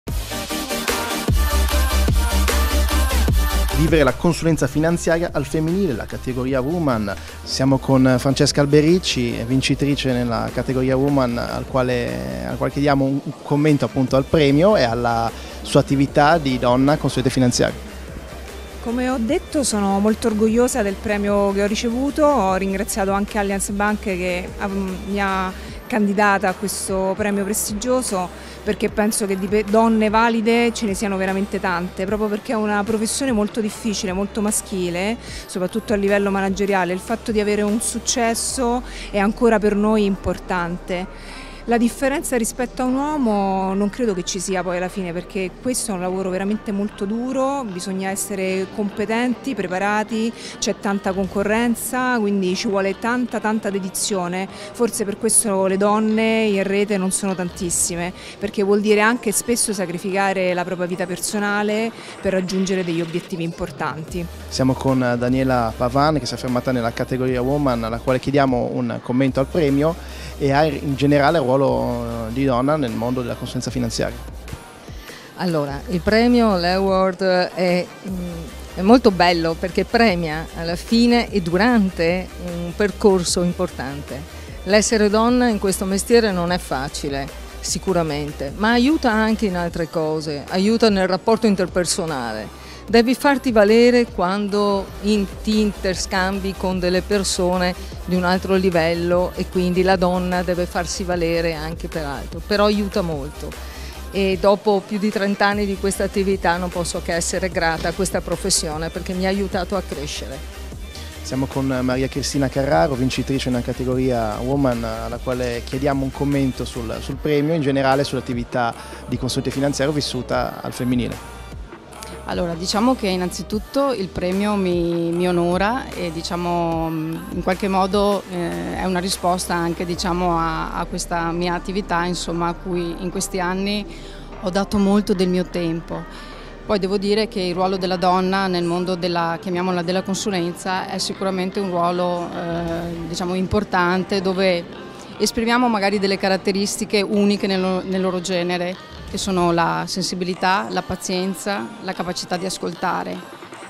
Le donne fantastiche della consulenza
L’estratto dalla nostra puntata di Bfc Events dedicata ai Bluerating Awards contenente le interviste alle vincitrici della categoria Woman.